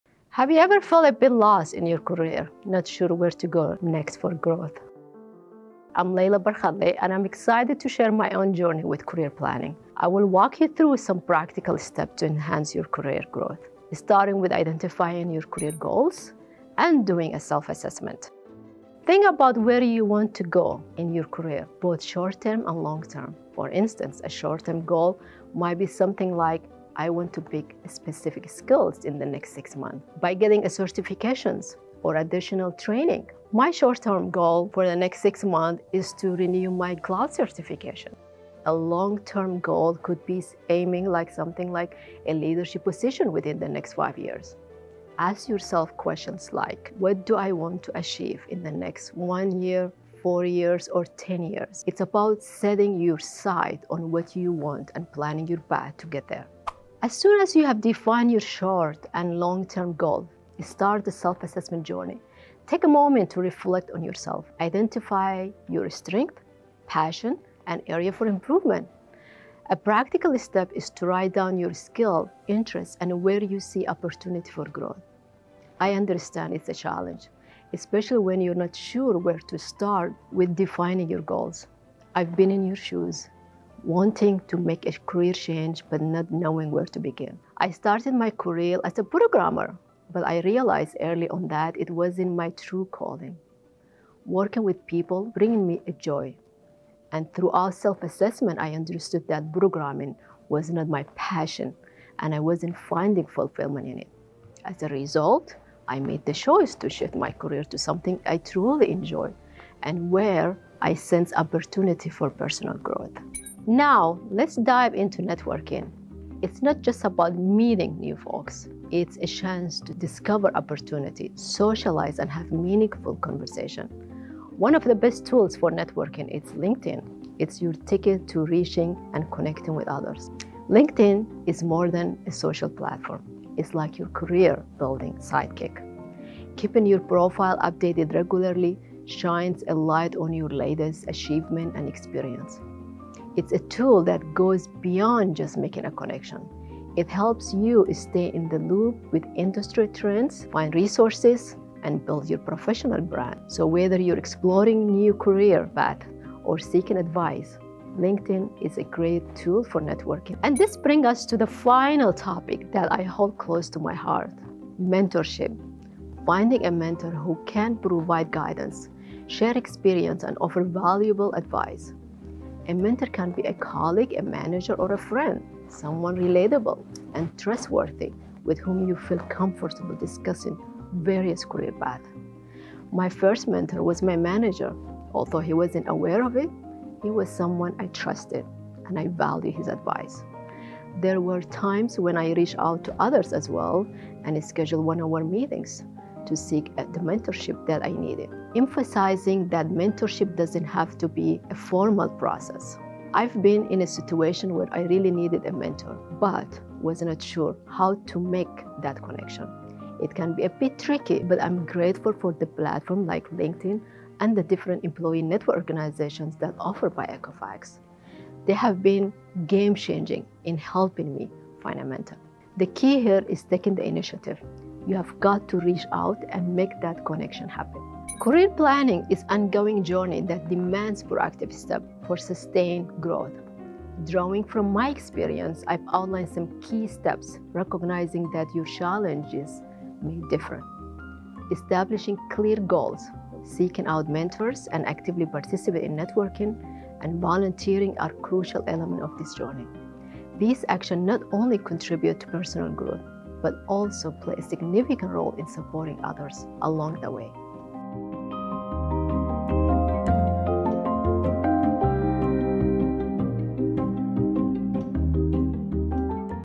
Career Planning 101 from a Women in Tech Winner
Leila Barkhadle, a passionate career growth advocate and mentor, WIT winner, and Sr. Director of Security and Compliance at Equifax, draws from her unique personal experience to offer guidance on growing professionally and shaping a career plan.